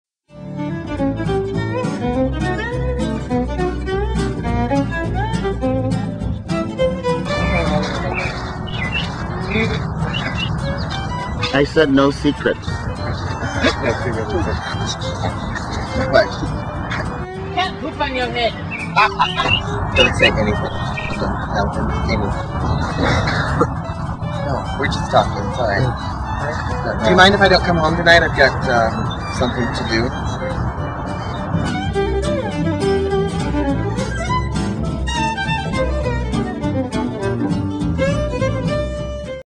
Sculpture Gardens 7: "Secrets"
This is 7th in a series of 9 featurettes I made when I lived in Minneapolis. Some friends came to visit and we went to see the Sculpture Gardens in Minneapolis. I took my video camera and had some fun. Everything was impromtu nothing was scripted.